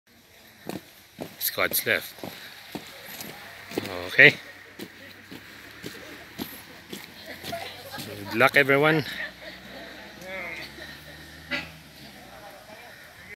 Touring PMA Cadets returning to barracks
PMA Cadets going back to barracks from touring.